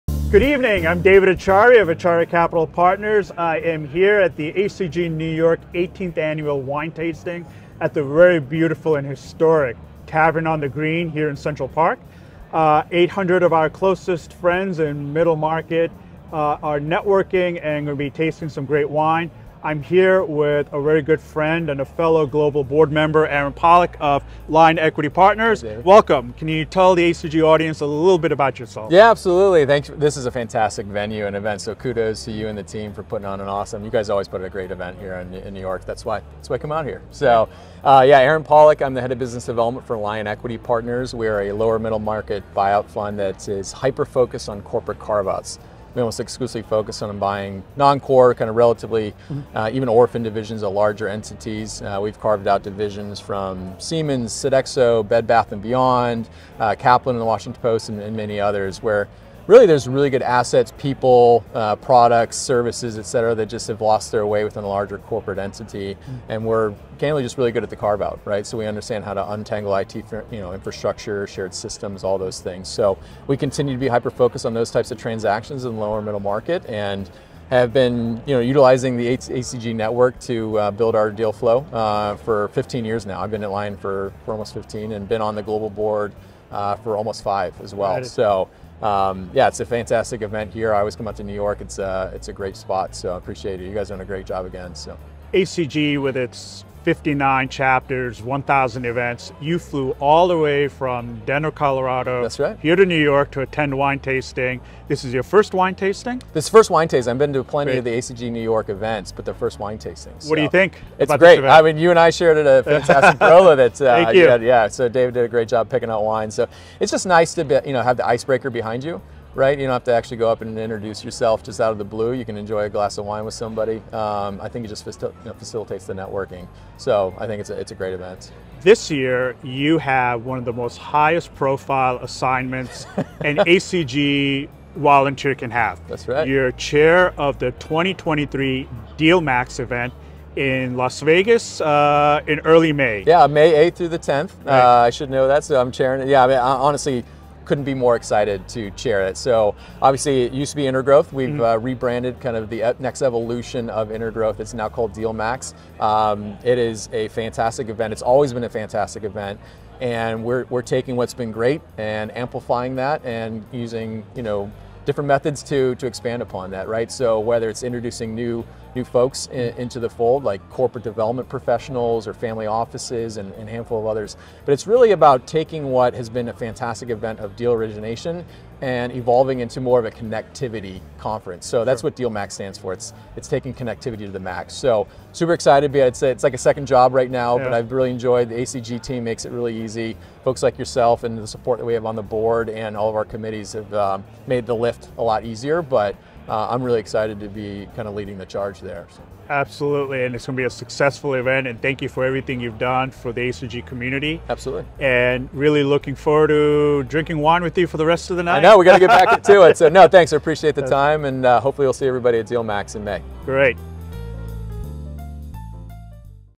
ACG NY 18th Annual Wine Gala Interview - Aaron Polack
Aaron Polack from Lion Equity Partners sits down with ACG NY during Middle Market Week. The event brought together leading global middle-market dealmaking professionals to develop and enhance their dealmaking activities, strengthen their long-term relationships, and provide numerous opportunities for networking.